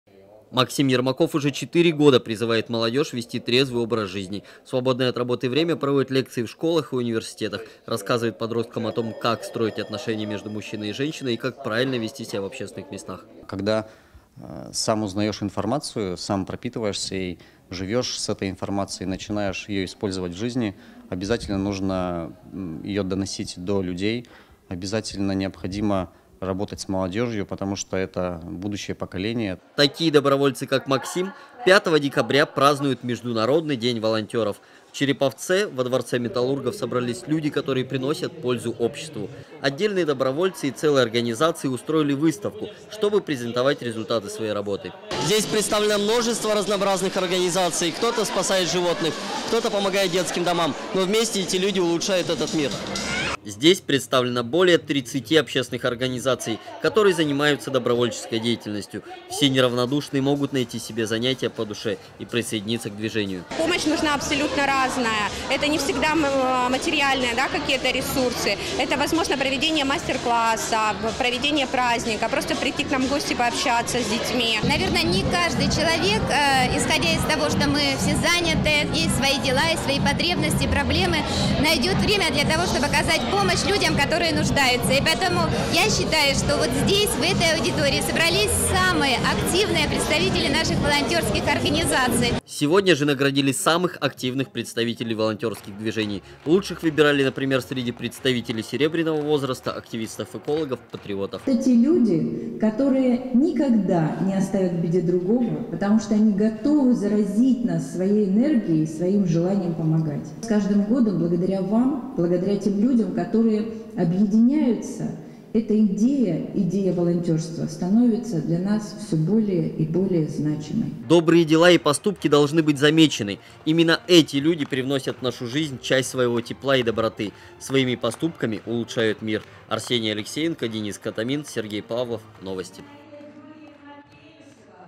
Волонтёрские движения Череповца представили свои проекты
Добровольцы Череповца собрались накануне в одном месте. Городской форум "Черволонтер" провели во Дворце металлургов. Таким образом в Череповце и по всей России подводят итоги года Добровольца.